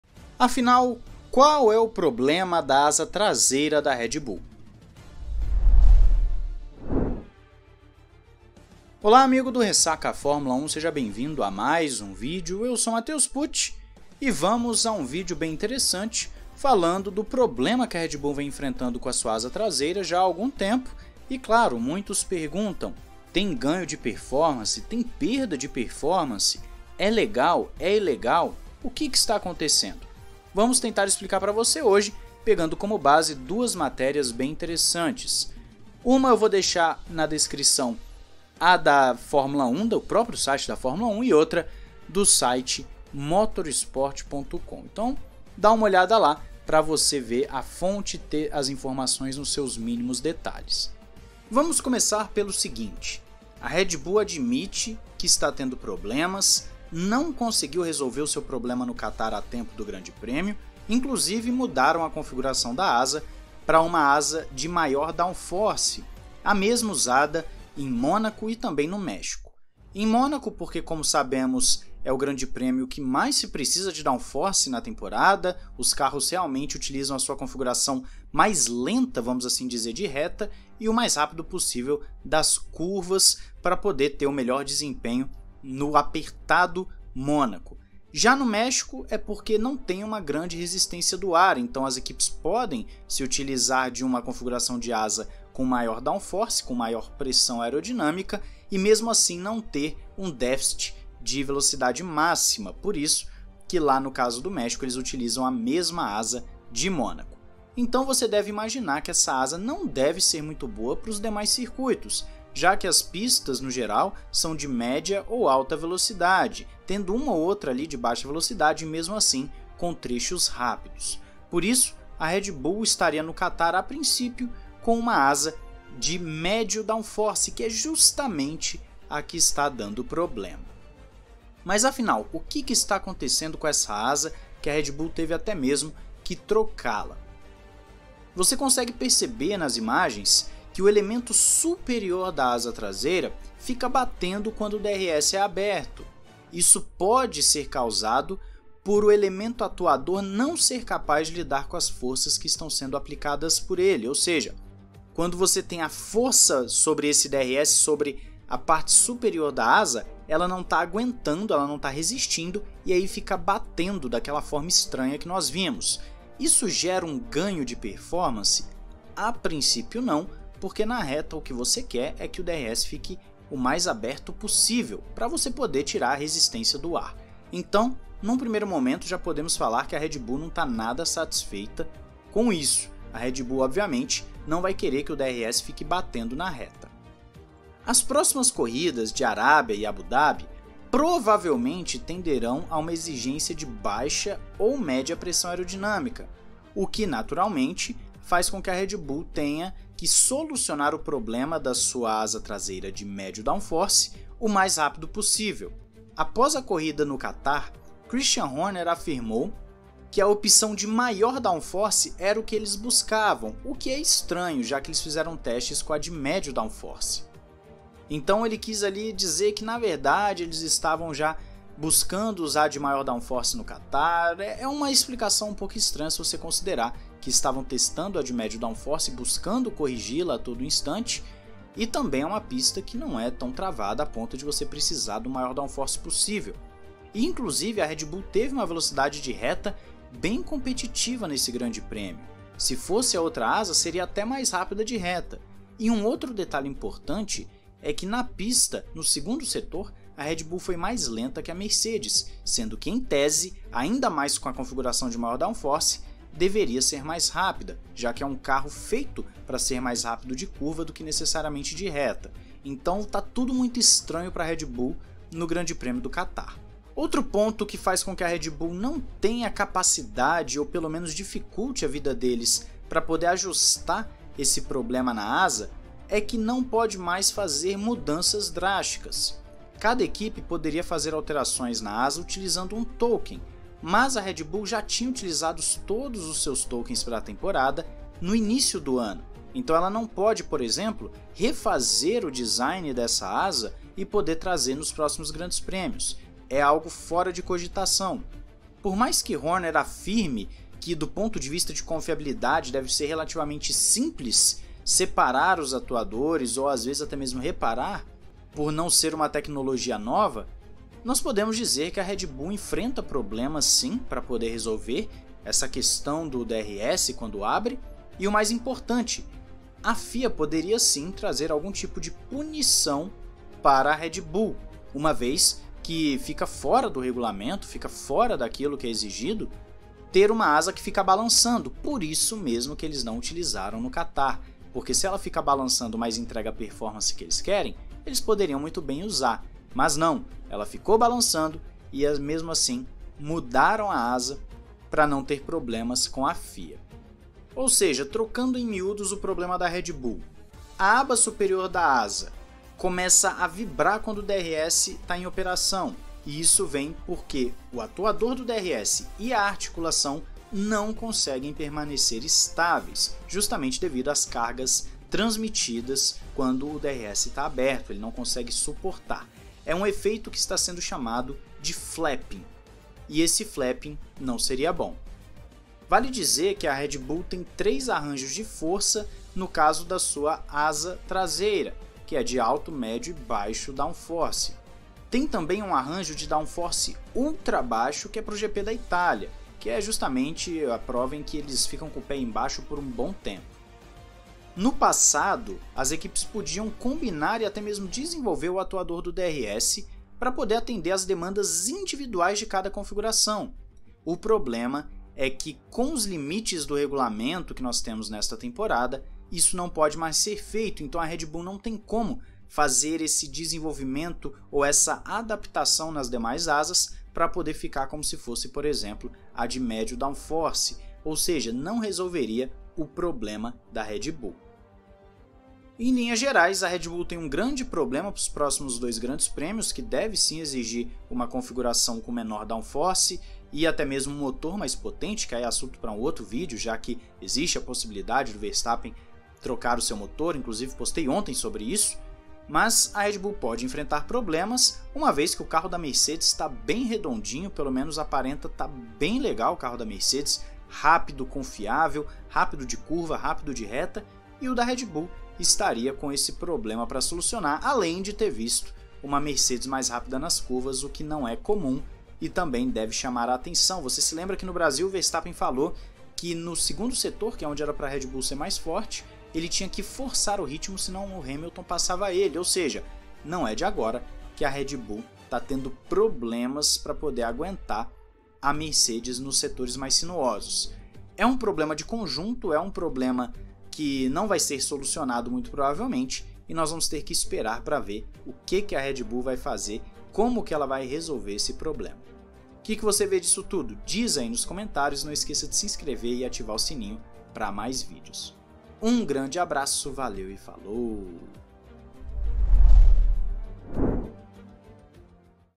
ENTENDA O PROBLEMA DA ASA TRASEIRA DA RED BULL
@scuderiamilani (Twitter)
YouTube.com/scuderiamilani

Gameplay do GP da Arábia por "Mitsos"

João 3:16